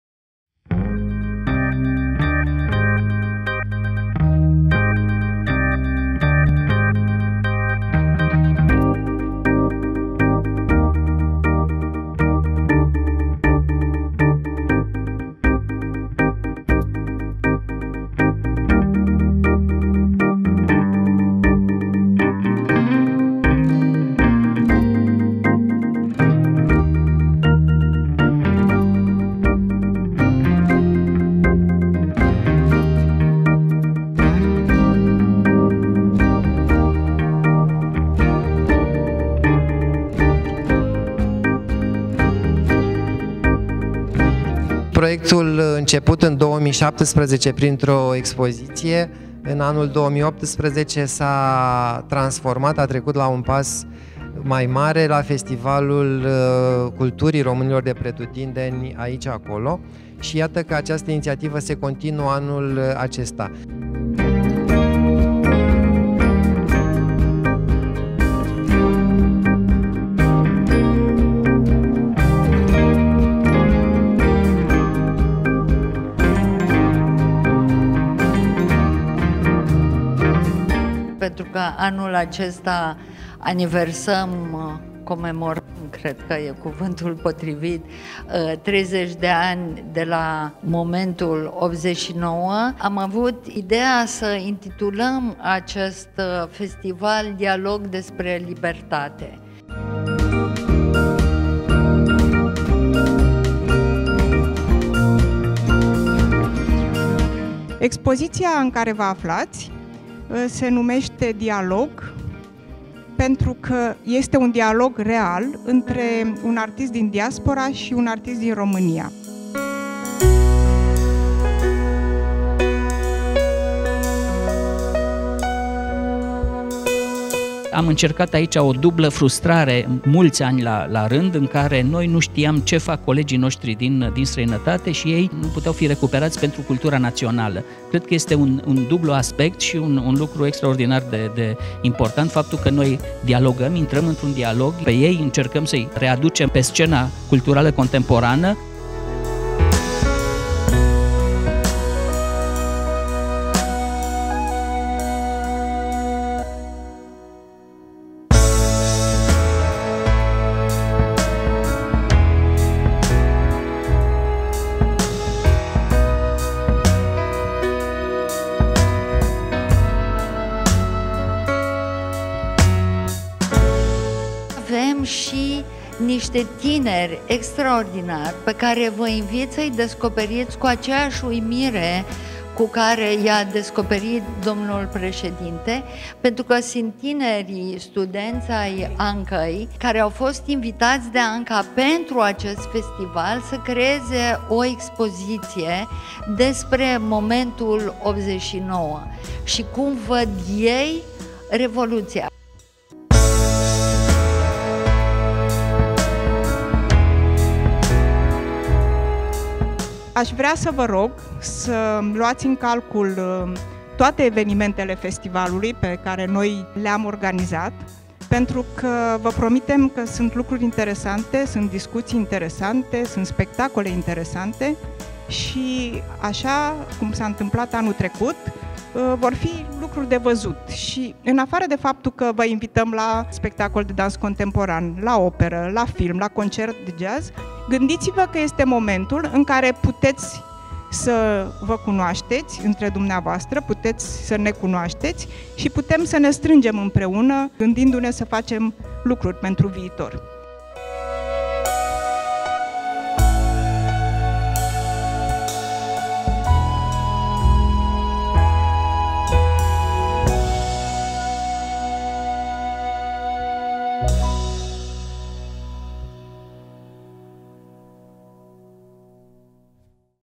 Festivalul Culturii Romanilor de Pretutindeni
Cu prilejul Zilei Romanilor de Pretutindeni, are loc la Bucuresti, in perioada 22-26 mai 2019, cea de-a II-a editie a Festivalului Aici-Acolo. Festivalul este organizat de Uniunea Artistilor Plastici (UAP), sub Inaltul Patronaj al Presedintelui Romaniei.

Festivalul cuprinde diverse manifestari care aduc impreuna artisti romani din tara si din strainatate, activi in varii domenii de creatie: pictura, sculptura si design, muzica, literatura, film, dans si opera.

Festivalul culturii romanilor de pretutindeni s-a deschis cu expozitia de arta vizuala Aici-Acolo de la Muzeul National Cotroceni, in care expun lucrari 40 de artisti:

Acest material va este oferit de Programul Catena pentru Arta.
Realizator: Bogdana Contras